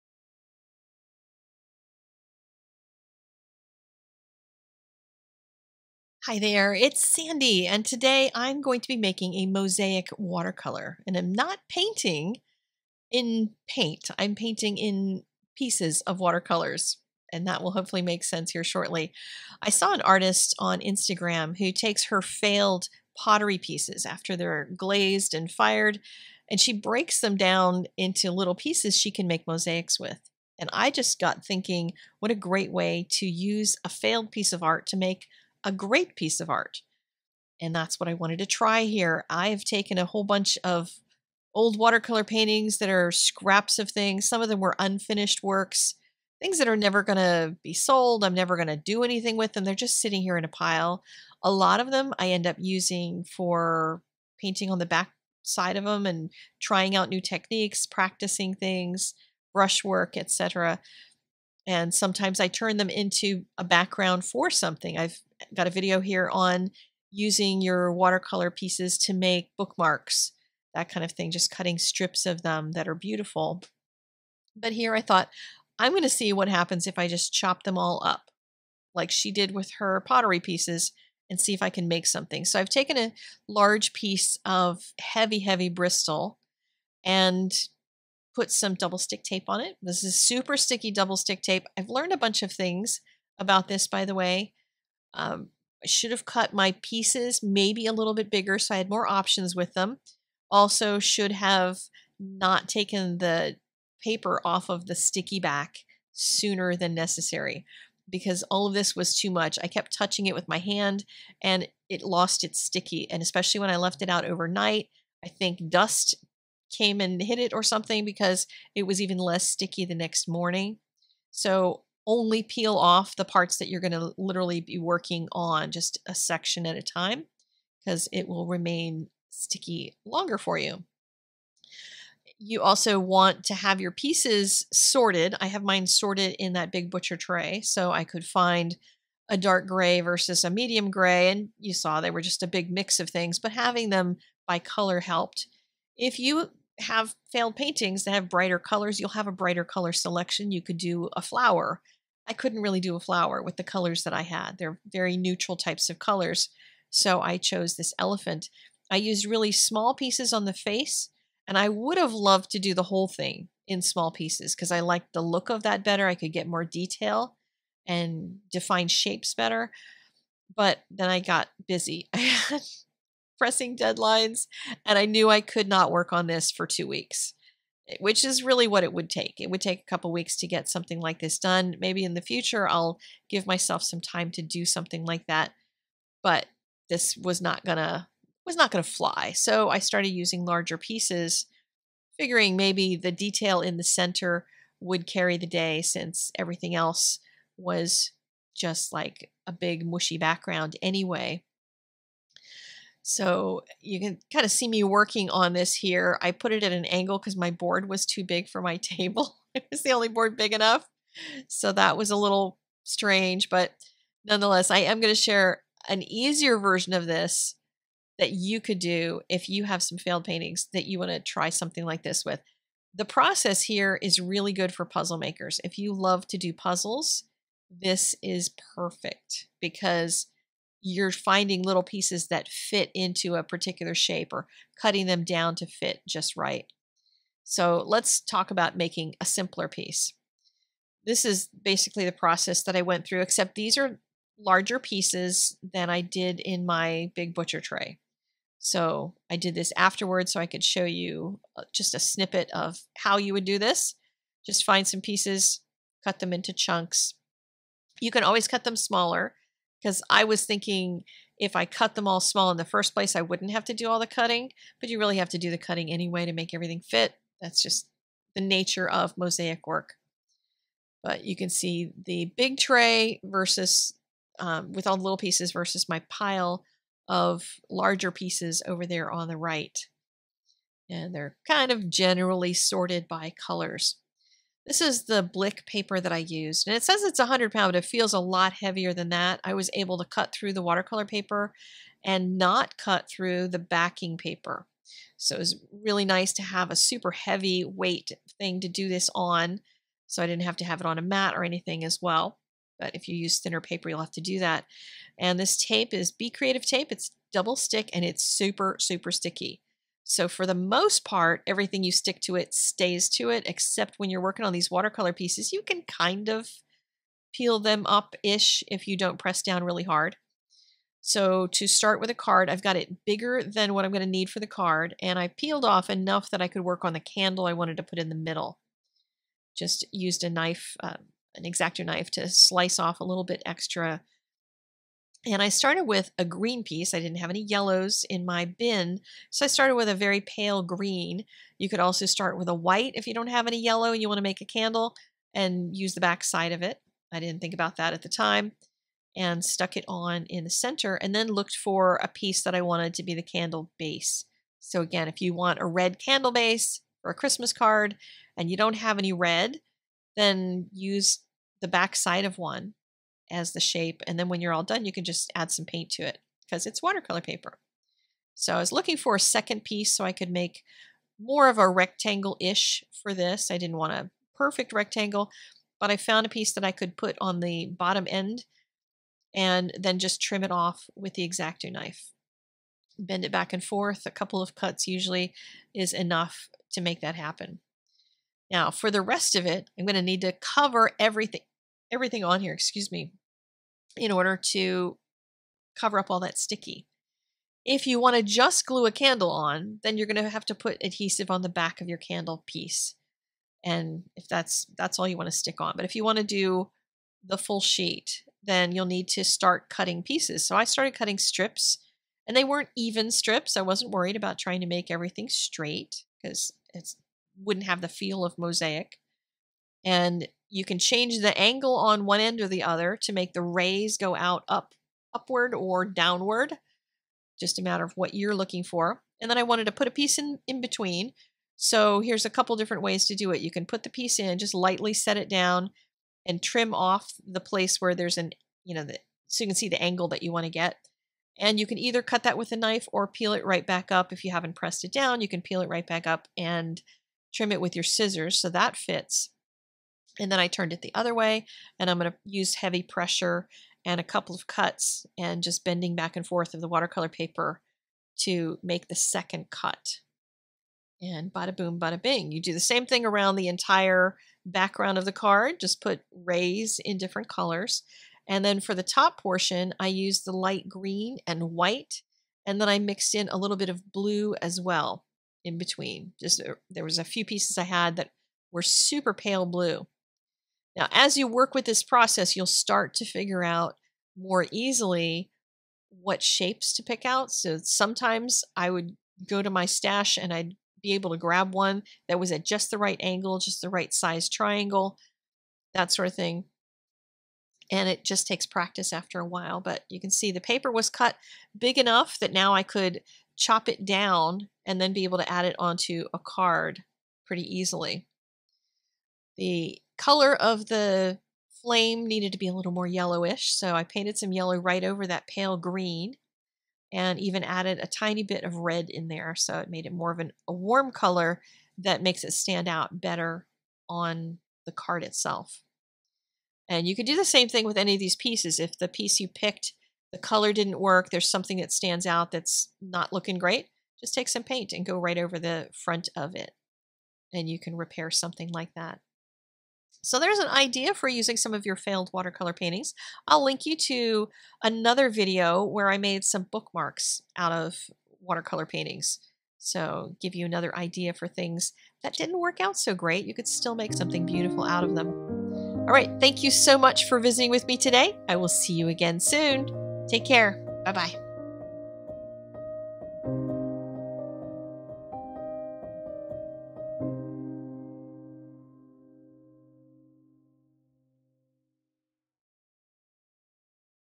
What to do with failed watercolor paintings: Watercolor Mosaic Technique
What to do with old watercolor paintings you don't like or abandoned? Blog post with more info and pinnable images

ScorTape:
2-1/2" wide
2" wide
1" wide
1/2" wide
1/8" wide